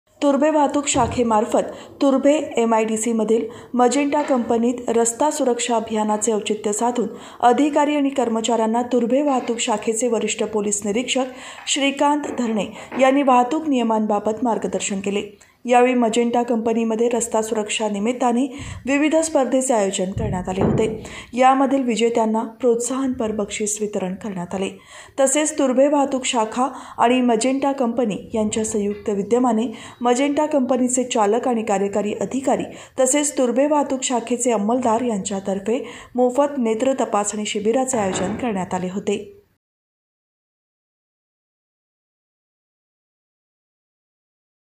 रस्ता सुरक्षा अभियान अतंर्गत मोफत नेत्र तपासणी शिबीर
तुर्भे वाहतुक शाखा व मजेंन्टा कंपनी संयुक्त विदयमाने रस्ता सुरक्षा अभियान अतंर्गत मोफत नेत्र तपासणी शिबीर